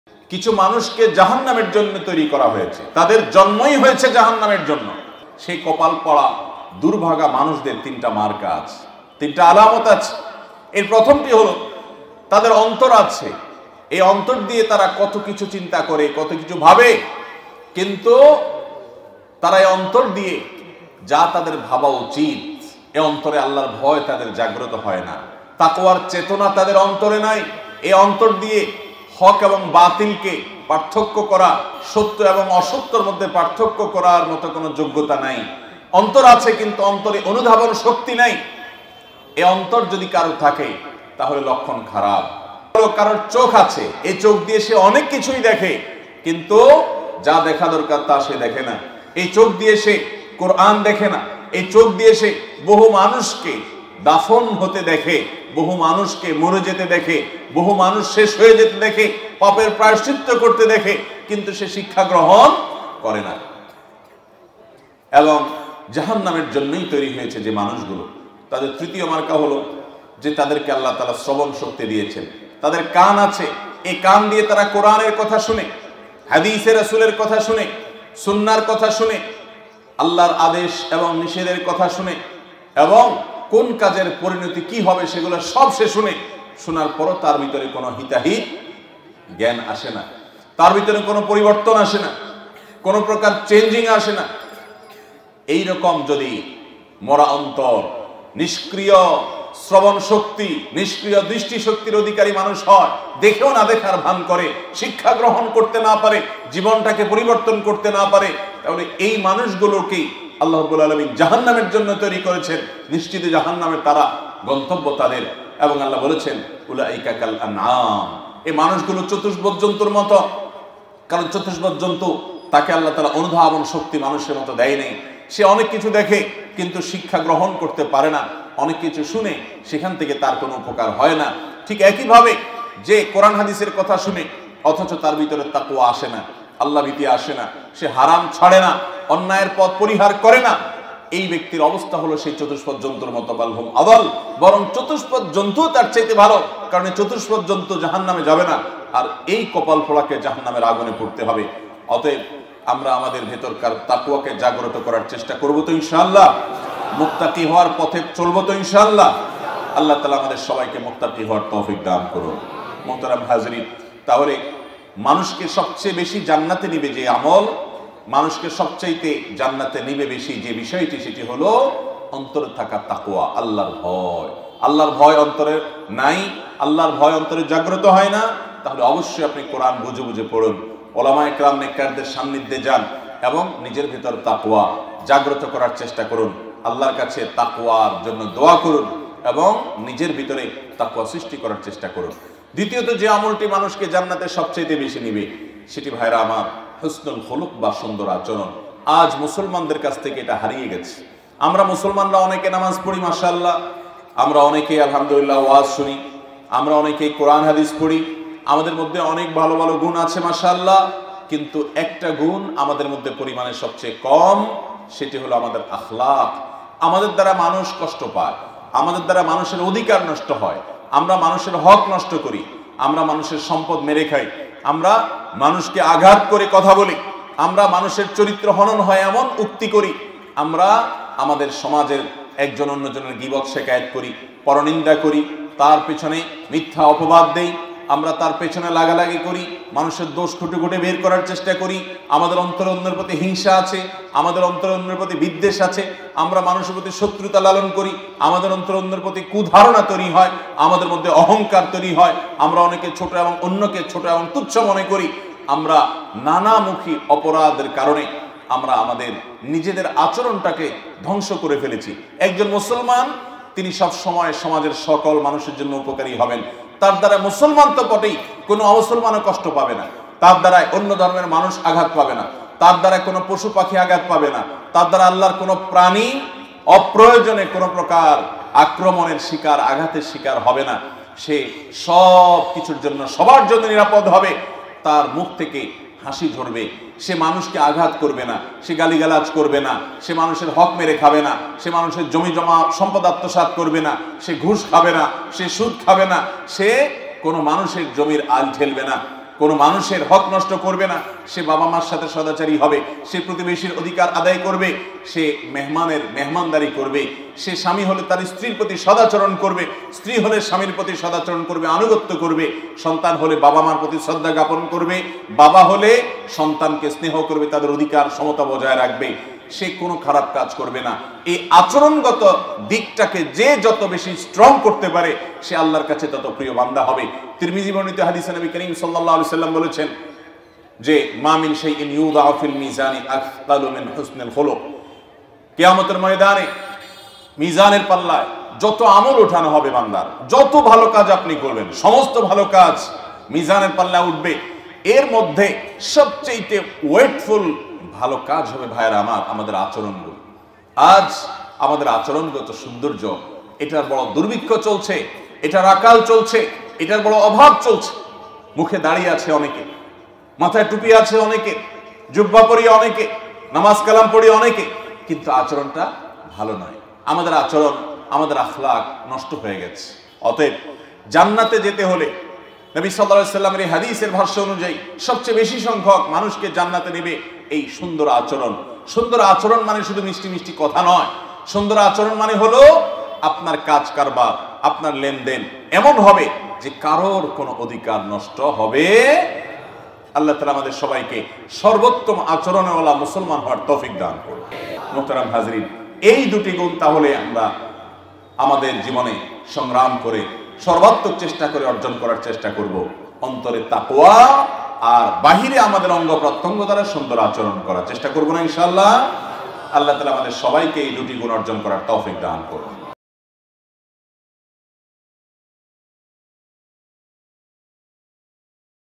জাহান্নামী মানুষের ৩ লক্ষণ ‍Shaikh Ahmadullah II New Waz 2022
আপনি জান্নাতের পথে চলেছেন নাকি জাহান্নামের পথে? ‍Shaikh Ahmadullah II New Waz 2022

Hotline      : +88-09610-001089